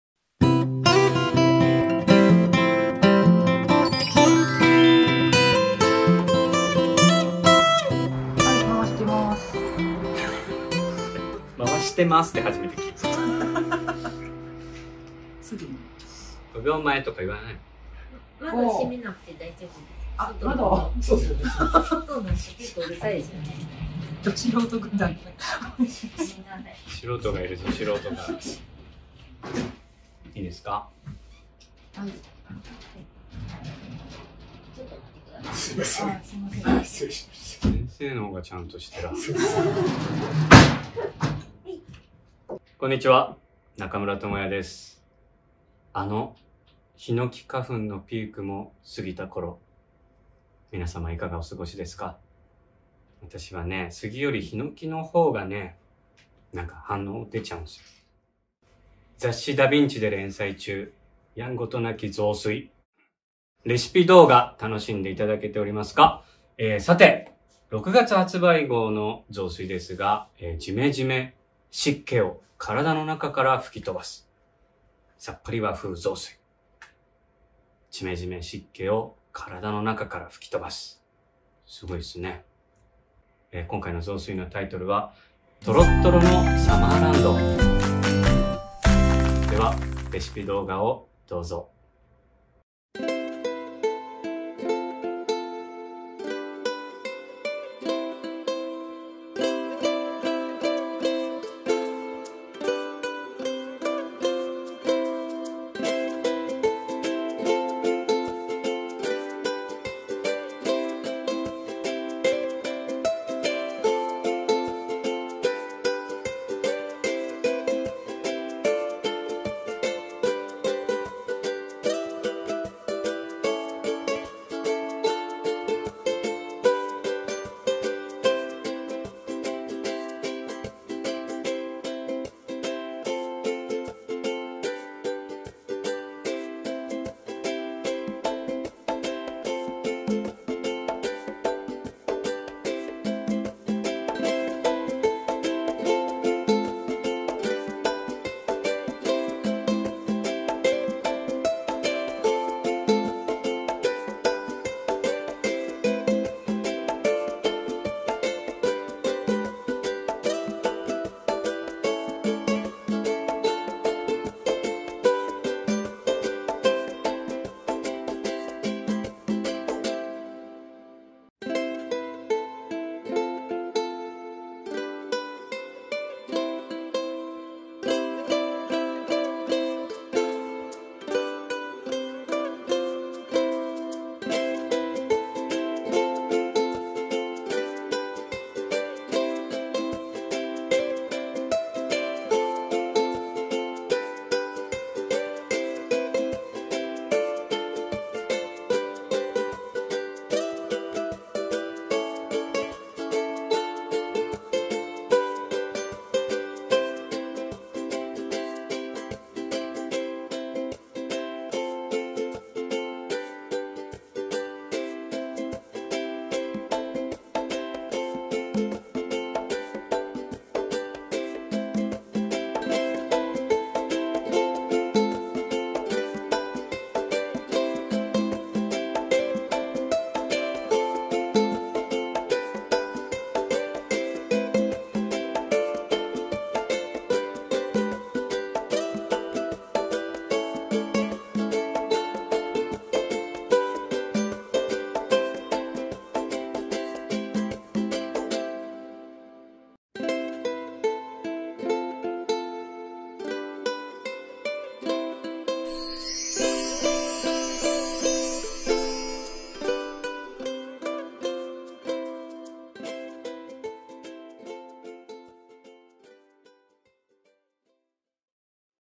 第4回【ハト麦茶を使う夏にぴったり雑炊】中村倫也さんと雑談＆雑炊レシピ公開「やんごとなき雑炊」
雑誌『ダ・ヴィンチ』（KADOKAWA）で大好評連載中！
俳優・中村倫也さんの連載「中村倫也のやんごとなき雑炊」誌面との連動企画。毎月おいしい雑炊レシピ動画を公開します。

●中村倫也エッセイ集「やんごとなき雑談」

●材料（1人分）
ごはん　1膳分（150g）
和だし汁（かつお、昆布）350ml
ハト麦茶（ティーバック）1パック（7g）
A（塩少々、しょう油小さじ1/4、おろししょうが少々）
長芋　55g
B（酒小さじ1/2、しょう油小さじ1/2〜1）※むくみやすい人は調味料を減らして薄味にする。